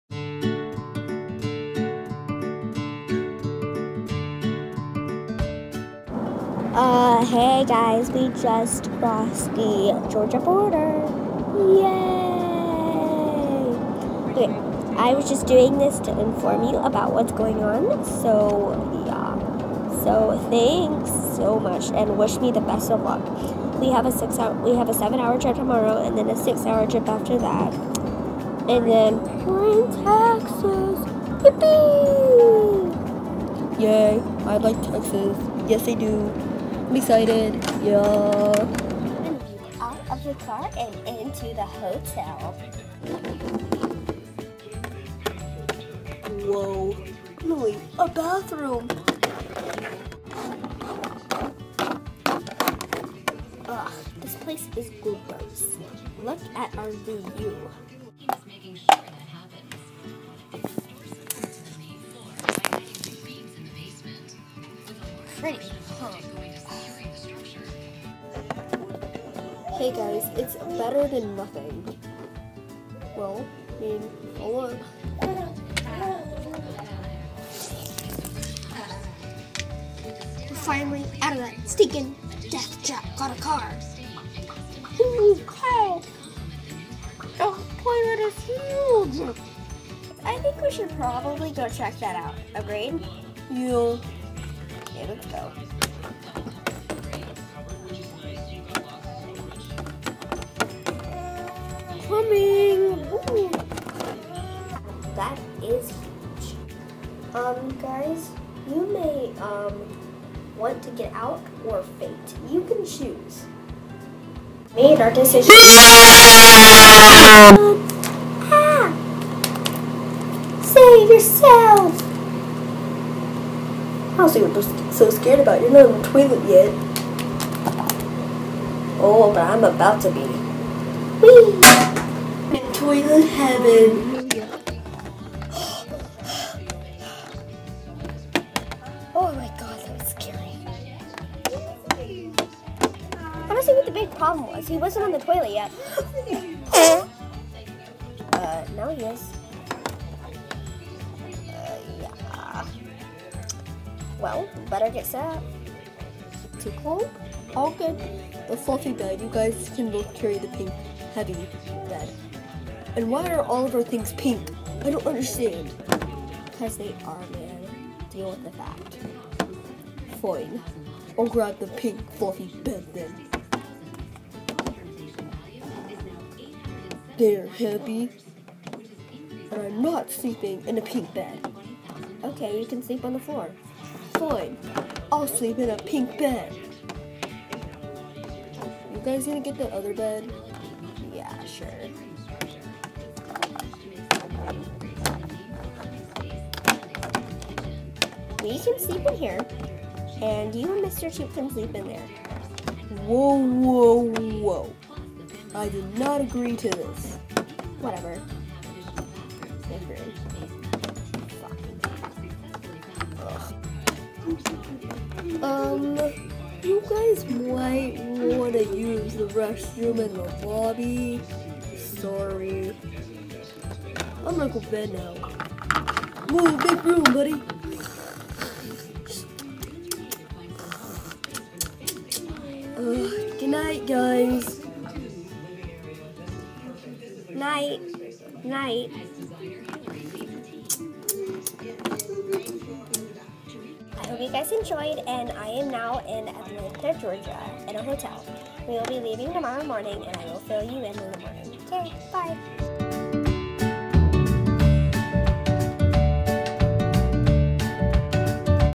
LPS: Texas Vlog Episode 1
If you hear the noise in the background... That's my mom and sister! Wish me luck on the rest of my trip!